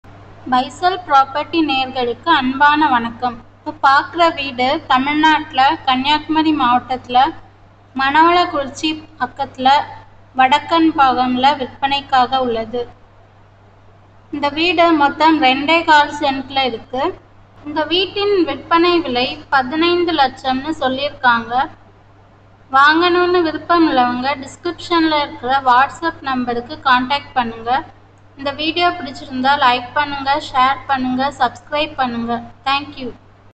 வடக்கன்பாகம் 2.25 சென்ட் வீடு விற்பனை Whatsapp:9626235532
மணவாளக்குறிச்சி அருகே வடக்கன்பாகம் 2.25 சென்ட் வீடு விற்பனைக்கு உள்ளது.
Bike  போகும்.வீட்டின் விற்பனை விலை 15 இலட்சம்.